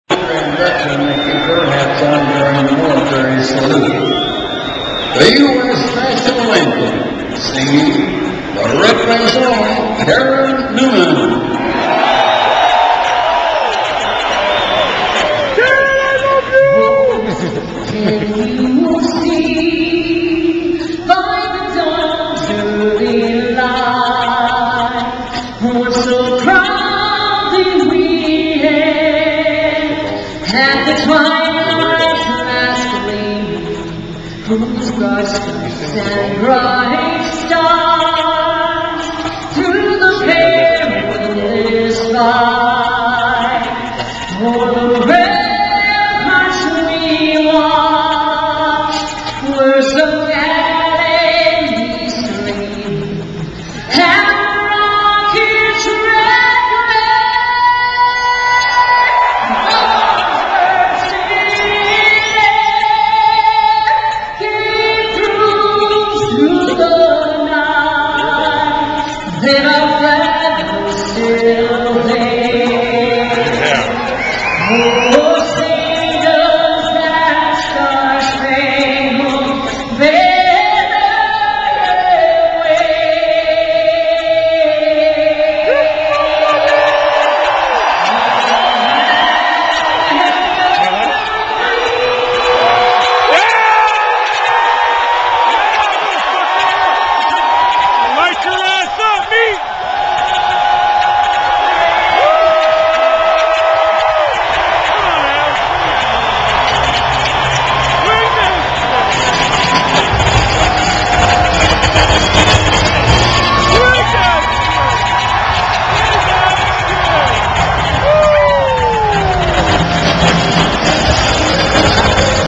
Karen newman and Hiller almost get the Octopus in the head.
wait till end of song and watch the octopus fly almost getting both of them. funny. and al swings the puss around.